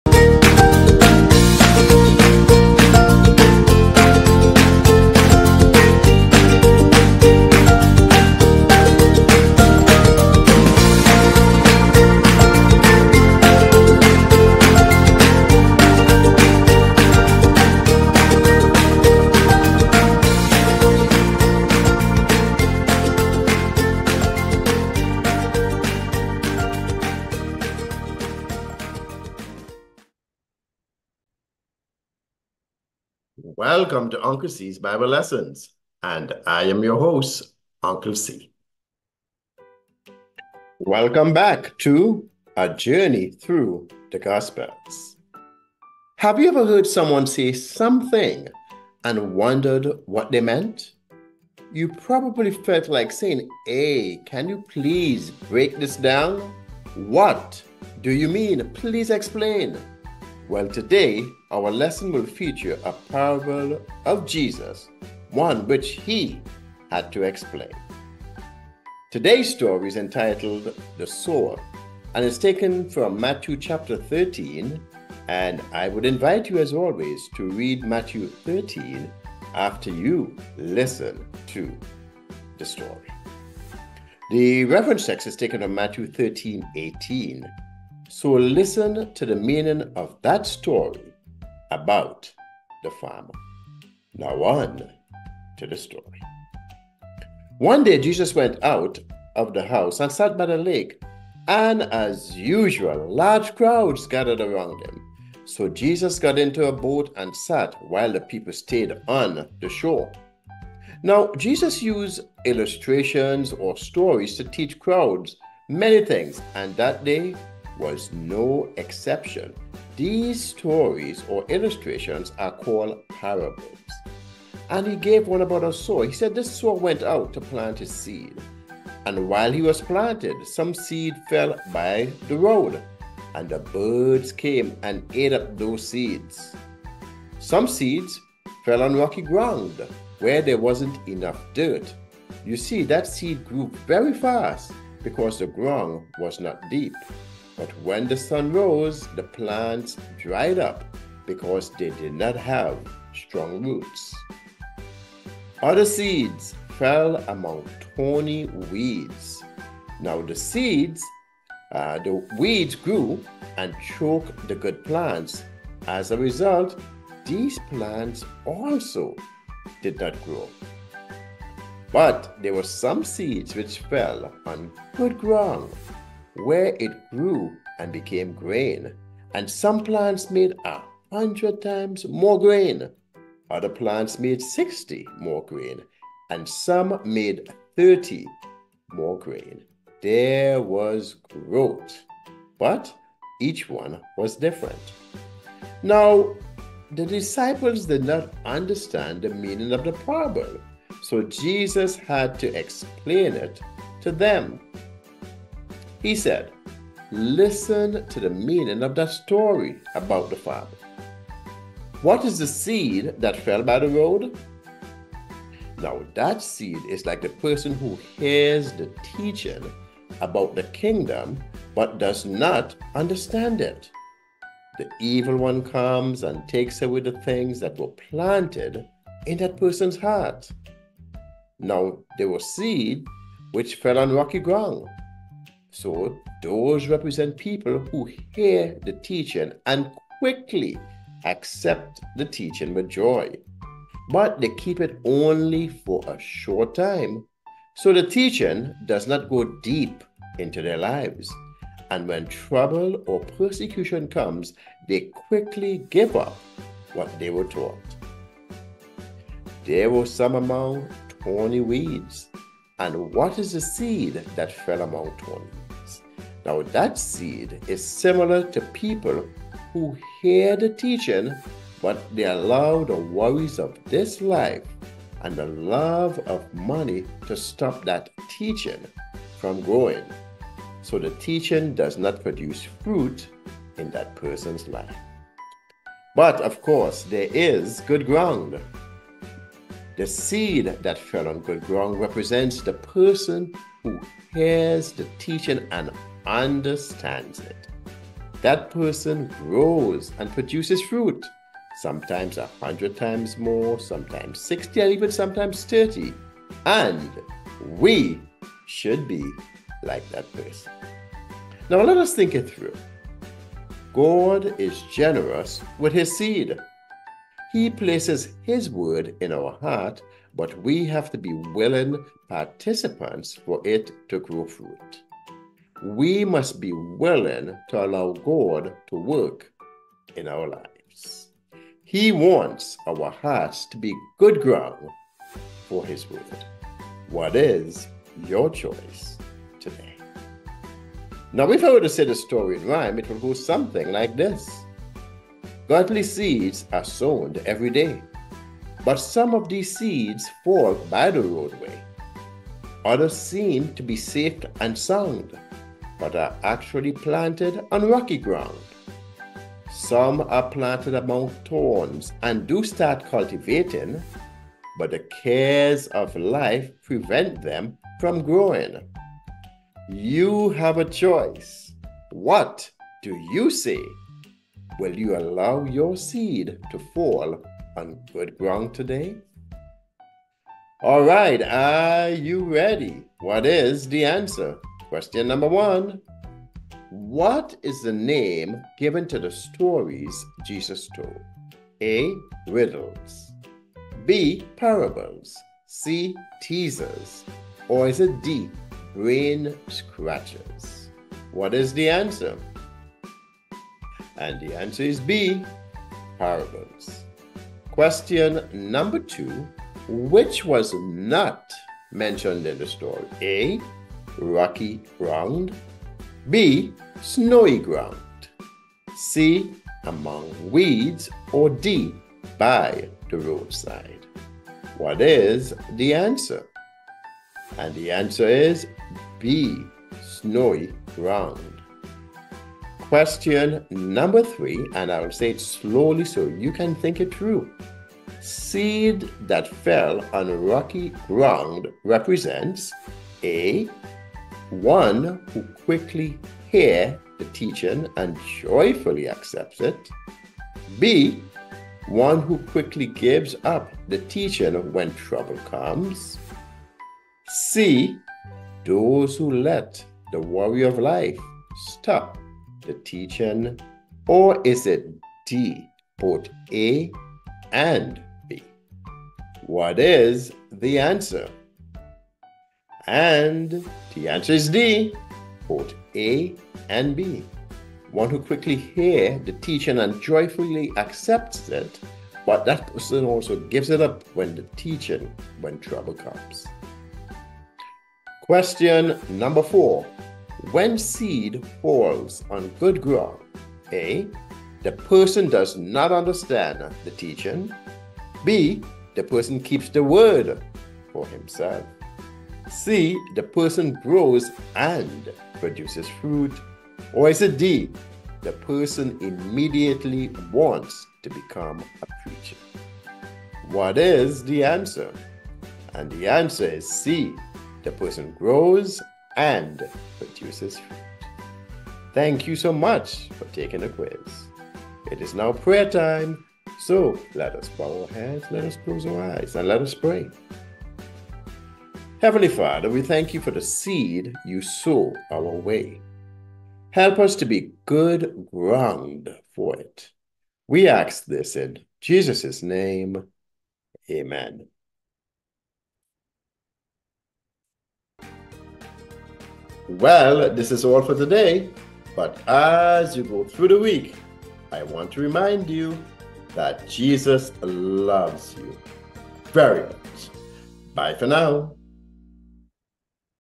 Lessons #220– The Sower
Bible Reading : Matthew 13

Bible Lesson for the young and young at heart.

I do not have the rights to them.